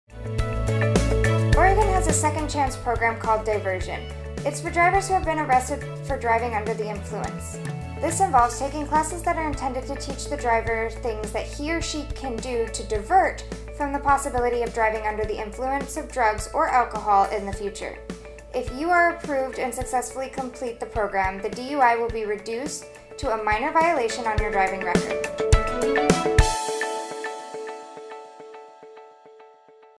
Diversion
If you are arrested and convicted for Driving Under the Influence (DUI) in the State of Oregon, the judge might deem you eligible for a rehabilitation program that lets you "work off" your conviction and reduce the charge to a lesser charge. It is usually offered to first time offenders. You still have to pay fines but can comply with the requirements by attending drug and alcohol diversion classes that you must pay for. You may still be required to install an interlock device in the car you will be driving during the probationary period. The good news is, you might not have to carry an SR22 once you have completed the courses and all the other requirements of the diversion and turn in your certificate and clearance from the courts to the Oregon DMV.

Talent: Felicia Delgatto
Produced, Written and Edited by Doug Hartley (Licensed Agent)
Co-produced and Edited by Sean Delgatto